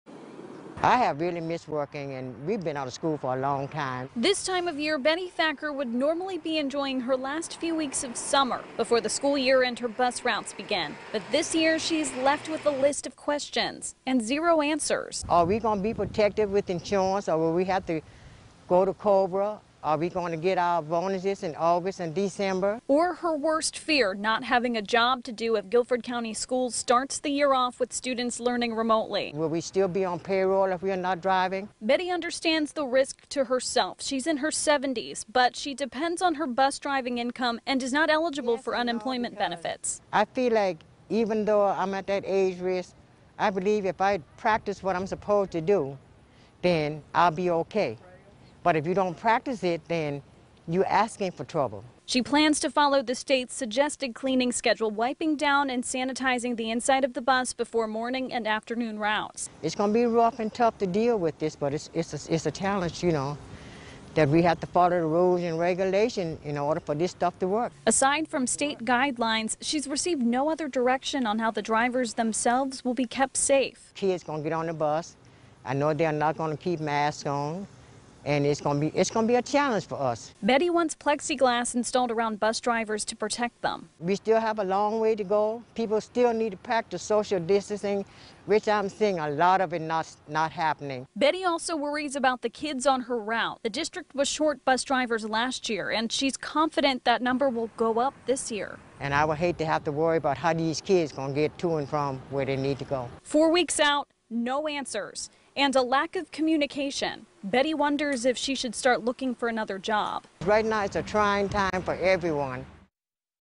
Local school bus driver concerned with safety and job security
They’re responsible for getting kids to school safely. As school districts across the Triad figure out what school will look like next month, bus drivers tell FOX8 they’re being left in the dark.